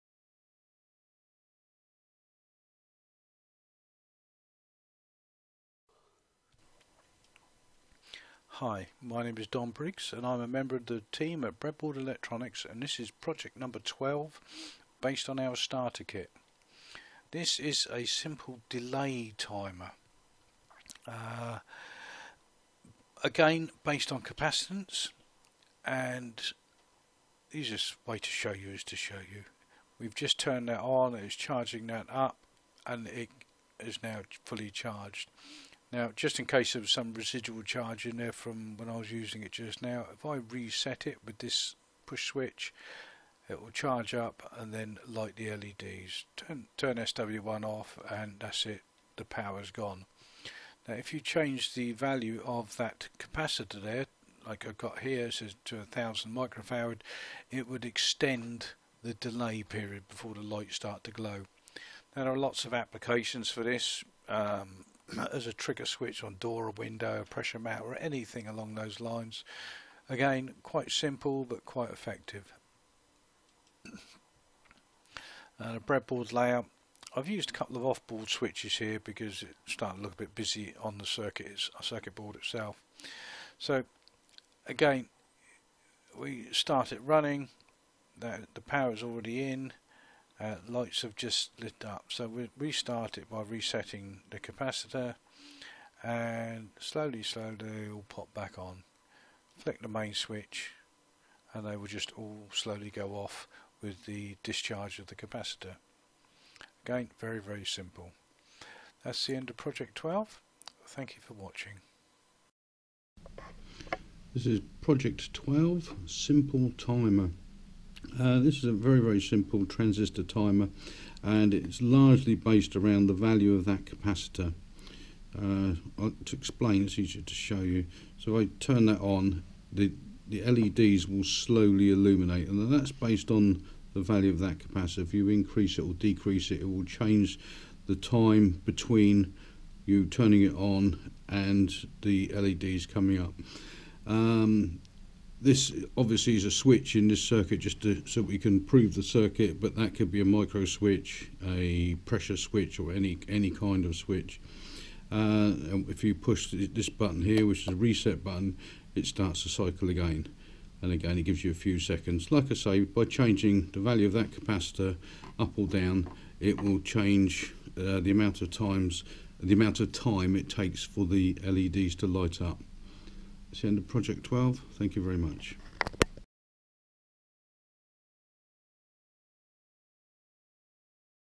Simple Capacitor Timer
Project 12: Simple Capacitor Timer.
Using a capacitor to store a charge, we can use this charge in an entry/exit type circuit.
No maths, no theory, just working circuits.